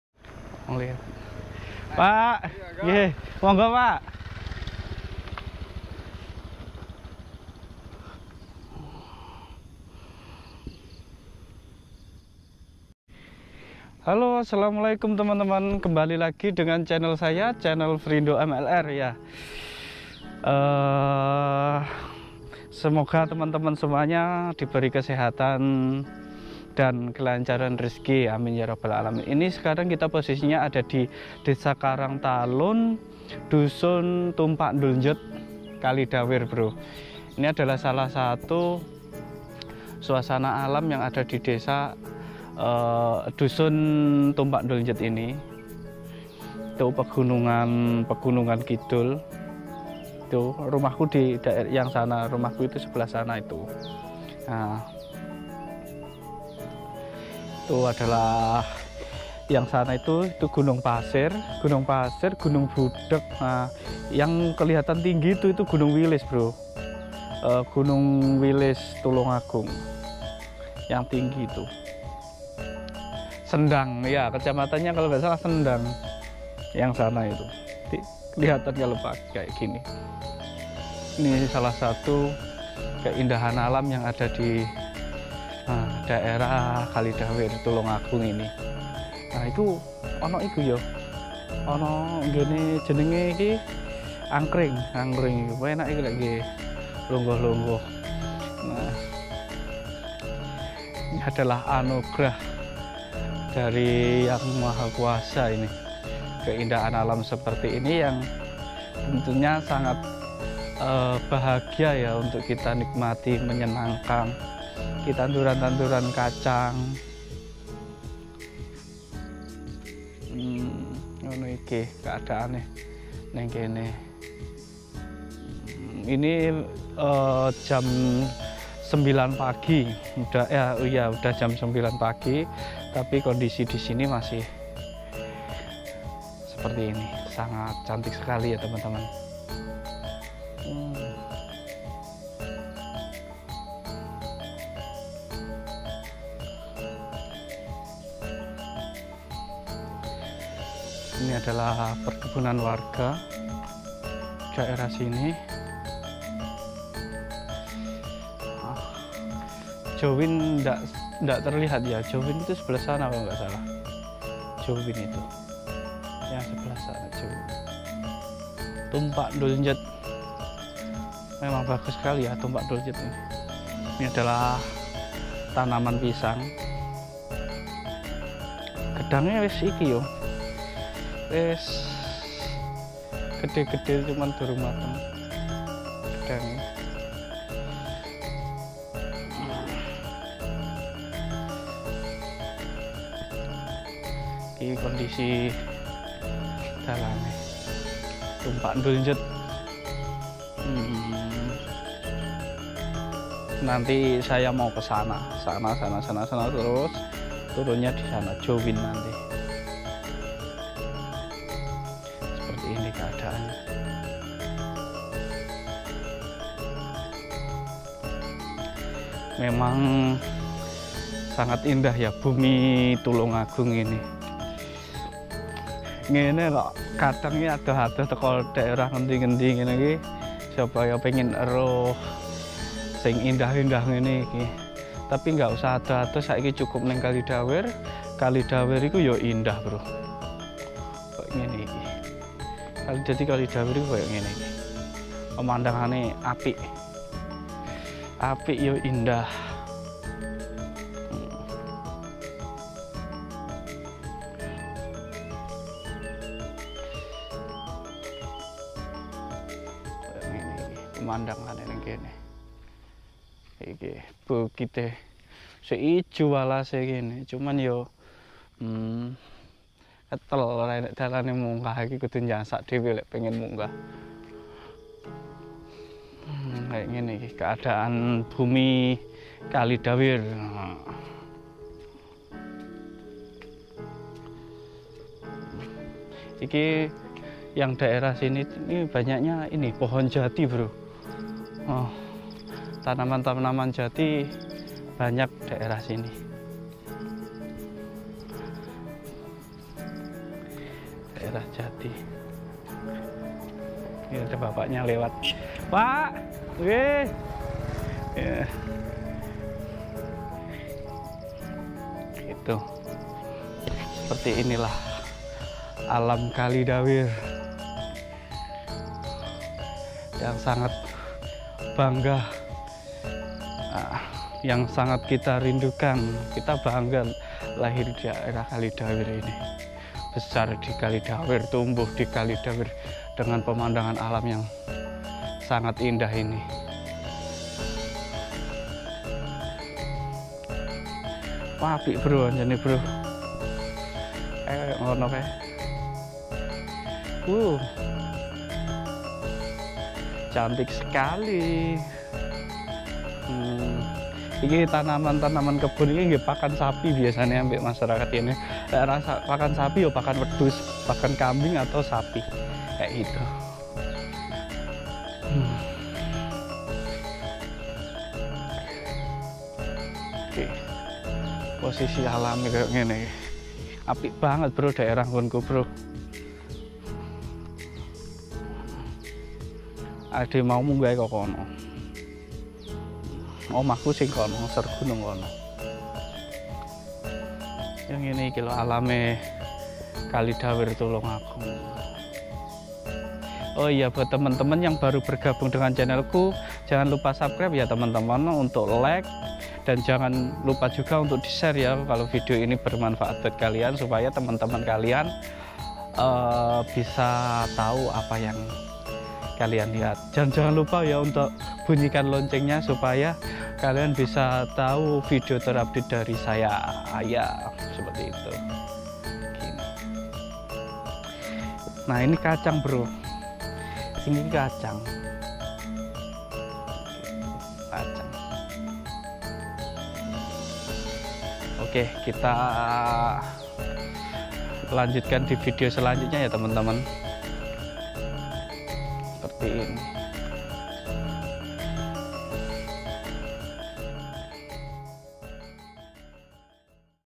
Pedesaan Bagaikan Lukisan Hidup
Tumpak duljet,Pedesaan Indah di Atas Bukit Tulungagung Selatan Jawa timur

Desa Karangtalun kec Kalidawir kab Tulungagung Jawa timur